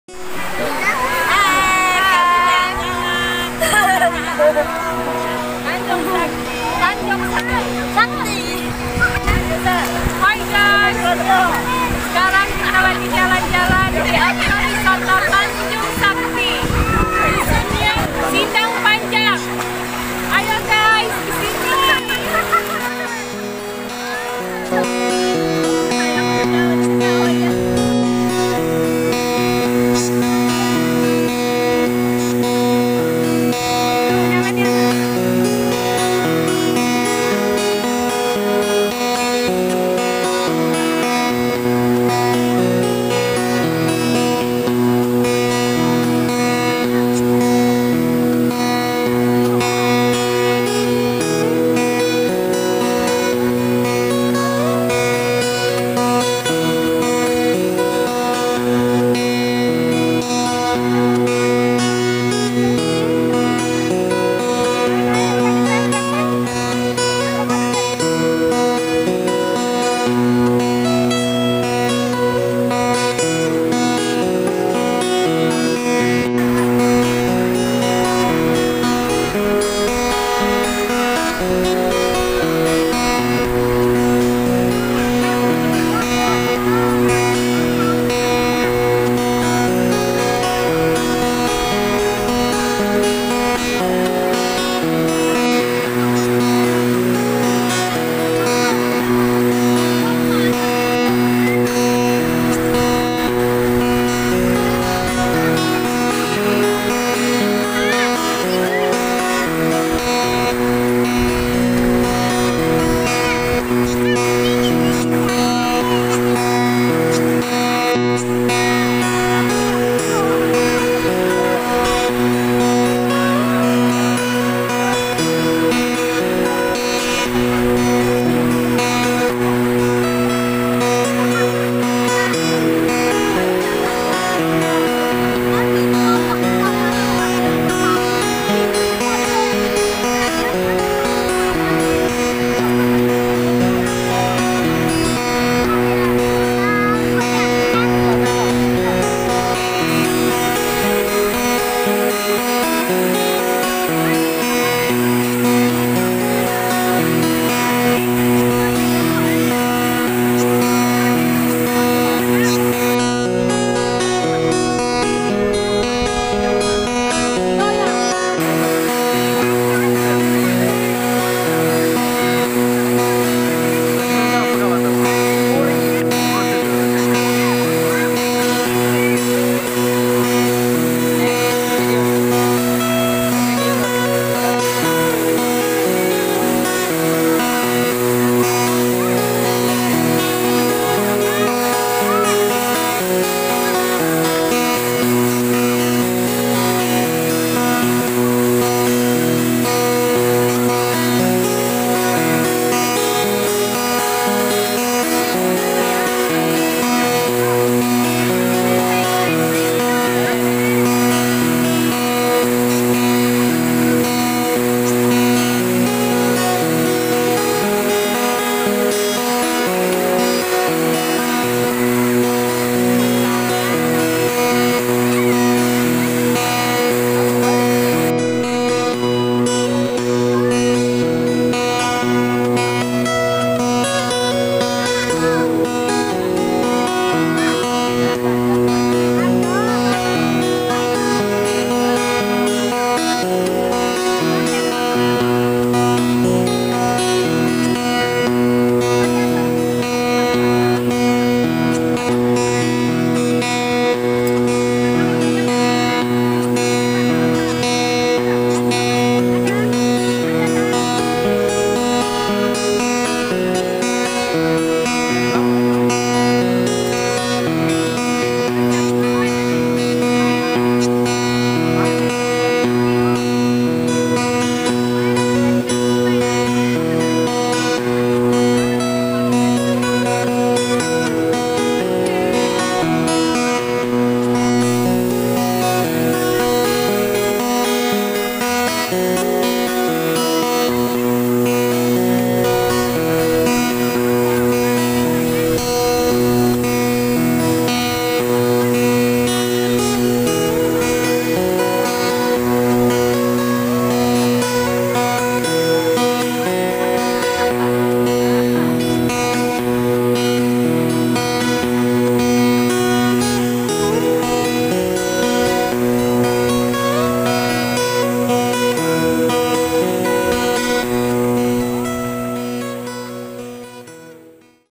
TEMPAT WISATA TERPOPULER DI SUMATERA SELATAN - TANJUNG SAKTI - KABUPATEN LAHAT - BESEMAH LIBAHG
Hai gaes kali ini admin, posting tempat wisata dulu ya, tempat wisata ini enak lho, udara nya sejuk, terus tiket masuk nya ngak ada alias (Gratis)
Kita hanya bayar tempat parkir saja gaes, dan itupun ngak mahal, hanya 5000 rupia per kendaraan roda dua maupun roda empat gaes,
Lokasi nya di sumatera selatan Tanjung sakti kabupaten lahat,
Di tempat wisata ini terdapat yaitu curuq yang bisa kita nikmati, terus ada kolam keluarga juga, yg juga bisa di nikmati sama anak anak,